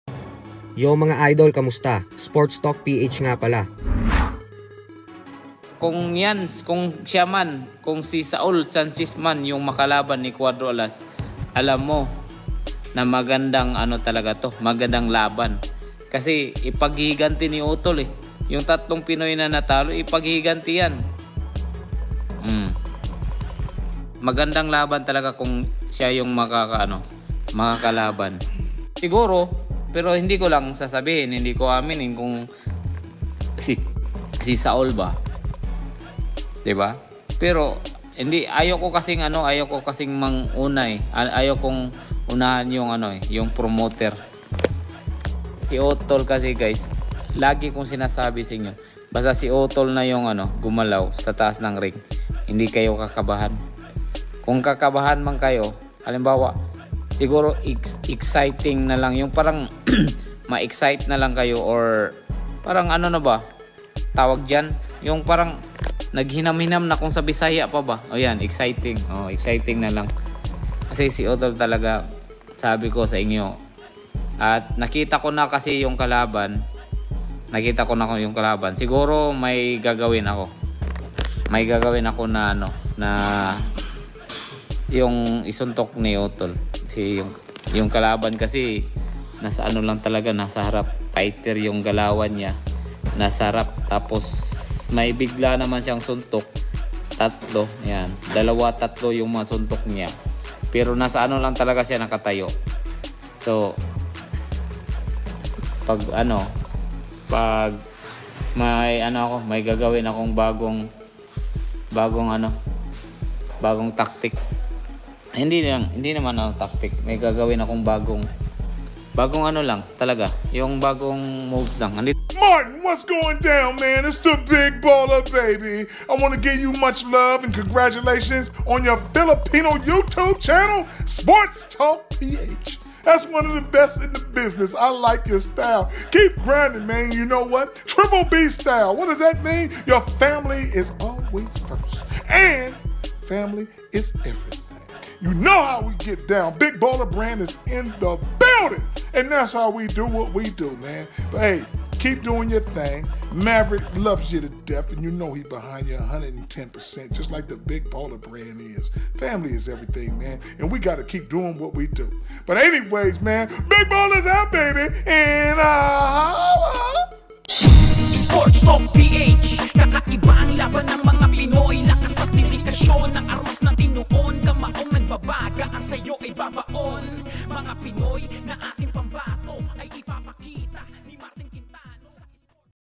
Casimero may TAKTIKA na para TALUNIN si Sanchez! I-GAGANTI ang 3 Pinoy na TINALO!
Boxing News & Updates!

Link po ng bagong Facebook Page natin: (SportsTalk PH) Pa-like at follow po mga idol!